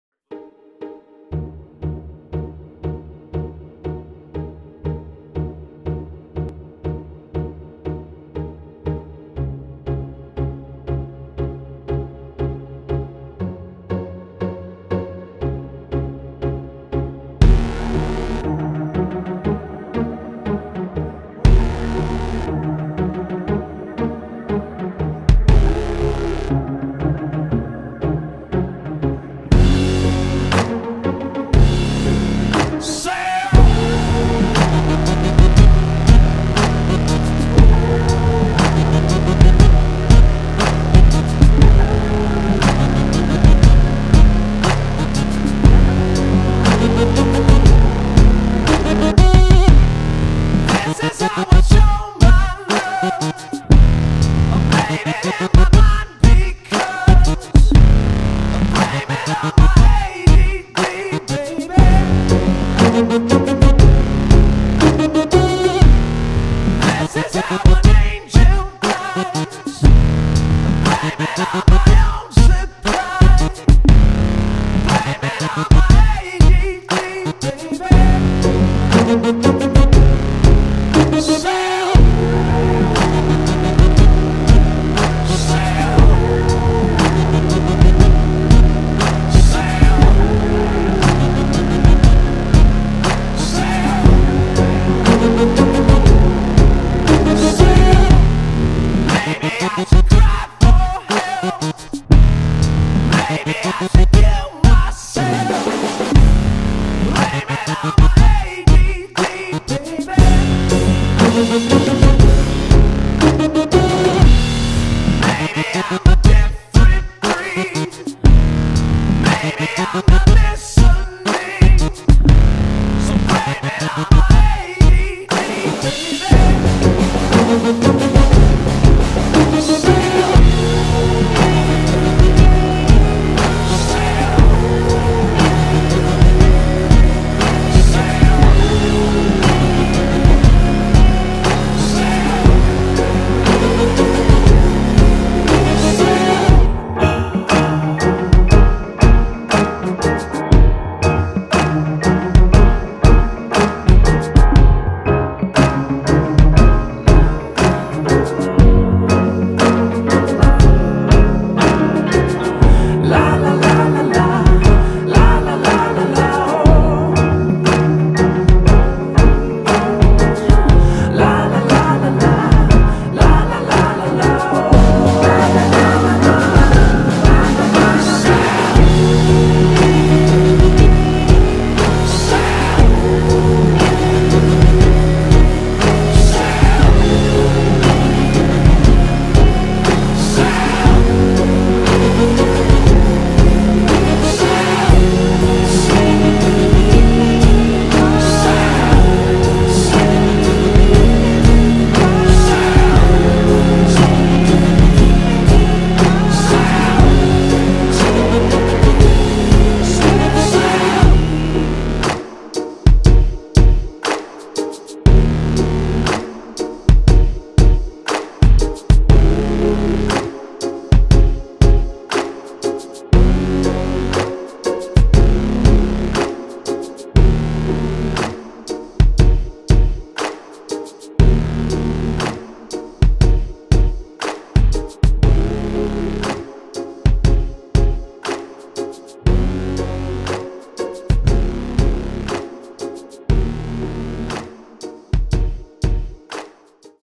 Red Bull Extreme Sports
Red Bull promotion video featuring bike stunts, skateboarding tricks, surfing, and the Red Bull space jump.